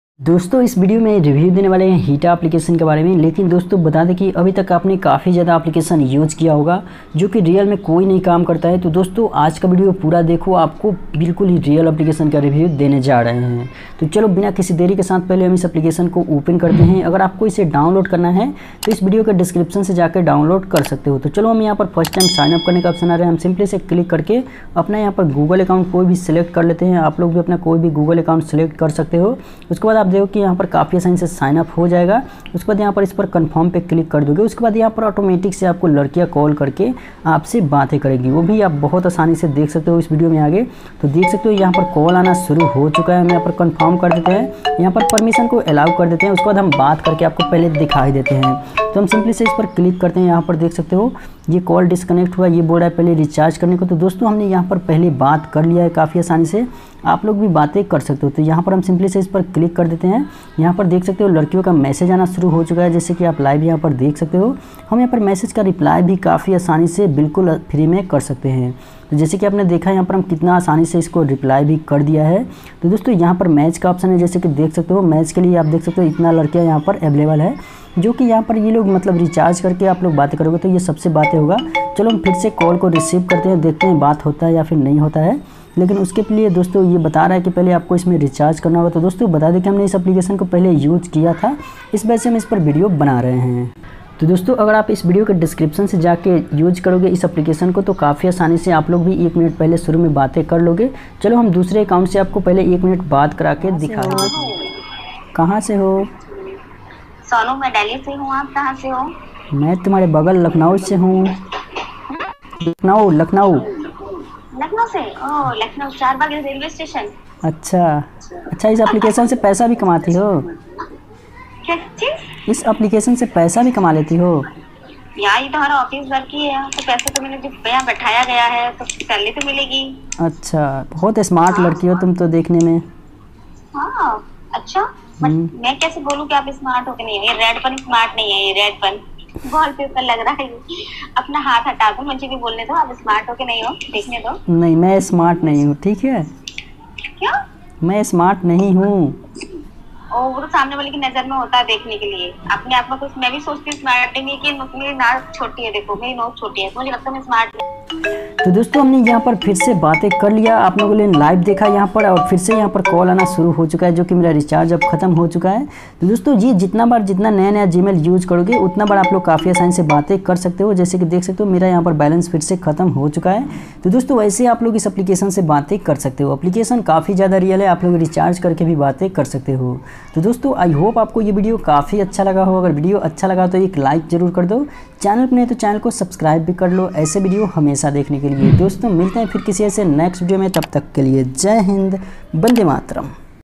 Online dating app 2022
नमस्कार दोस्तों स्वागत है आपके और हमारे चैनल में दोस्तों आज के विडियो  में बताने वाले है की आप online dating app kaise use kare

💠 Video editing and voice by :- Pintu Kumar

🔶 Videos Querys :-